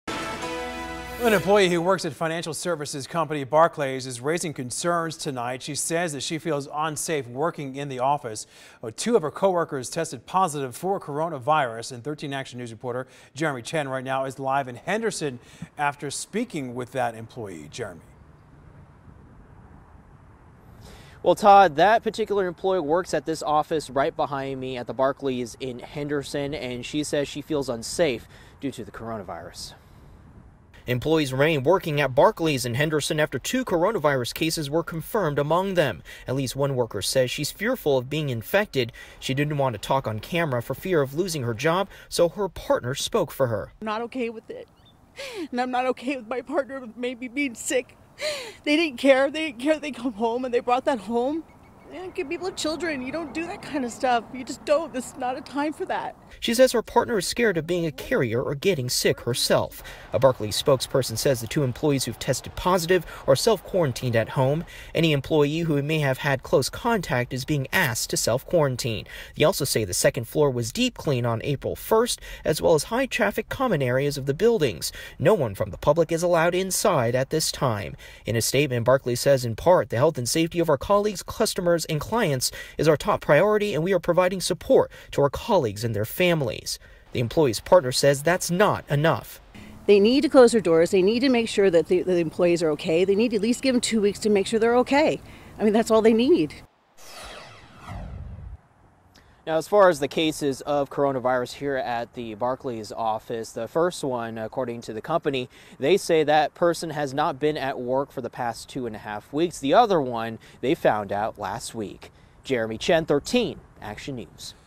Barclays employee raising coronavirus concerns
Workers inside the Barclays building in Henderson continue to work even after the company confirms two of its employees have tested positive for the coronavirus. Jeremy CHen reports.